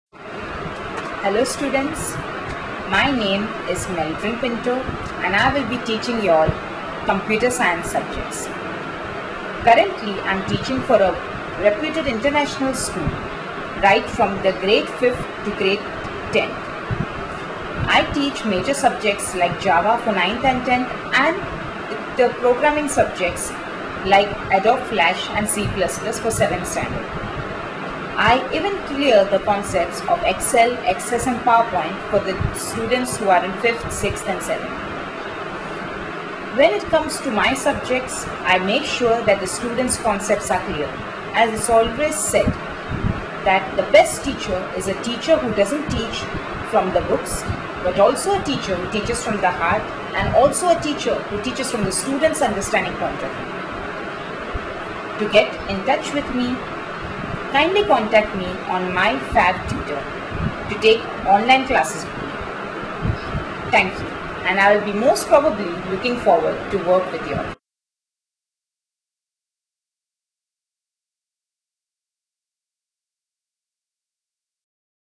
Meet Meldrin Pinto on MyFavTutor - Join Her Online Tuition Classes on Computer Science
Get connected with Meldrin Pinto Ma'am on MyFavTutor today. Join her online tuition classes to solve your problems and doubts related to the computer science subjects.